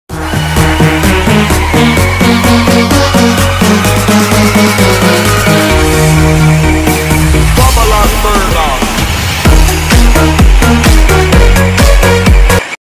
OMG😮🤩 Absolutely Unreal Driving Skills#Short
Like a boss driving skills from the world of amateurs, including rally car racing, amazing parallel parking while drifting! Inspirational drivers, saving trucks and big rigs, along with breaking world records!

please enjoy moreover skill and technology